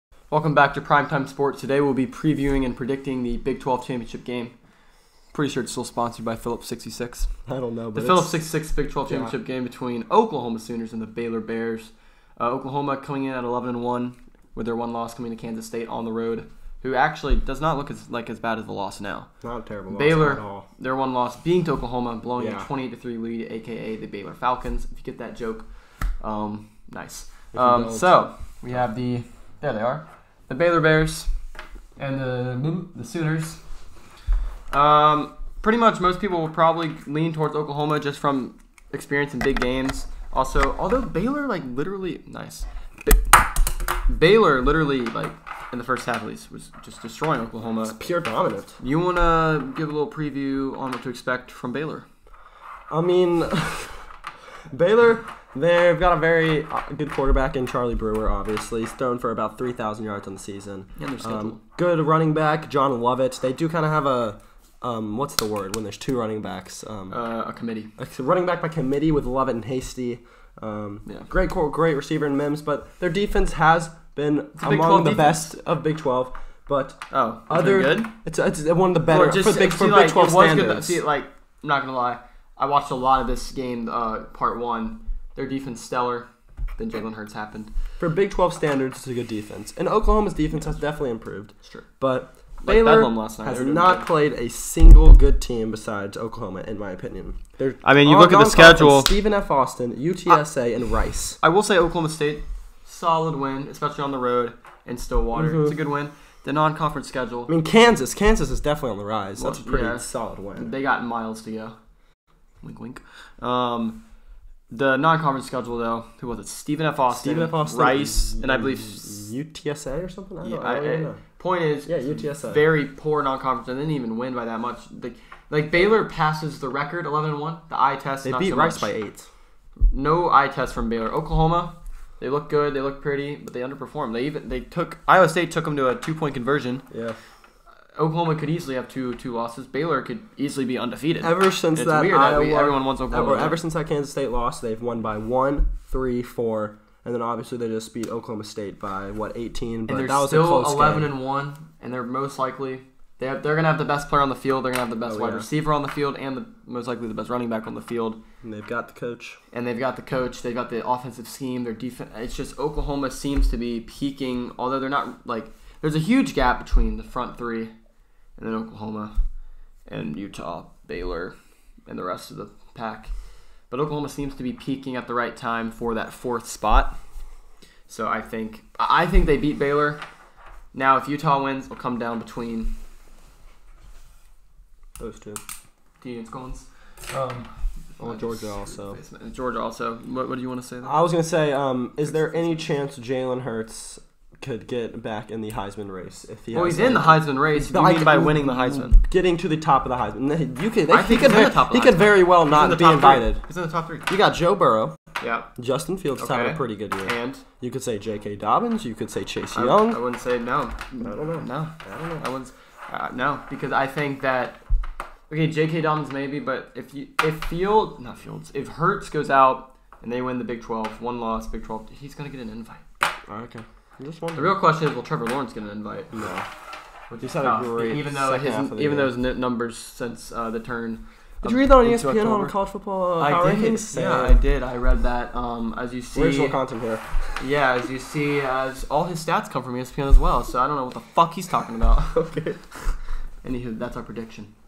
#6 Oklahoma vs #7 Baylor Preview and Prediction | 2019 Big 12 Championship
one more left... big 10!!!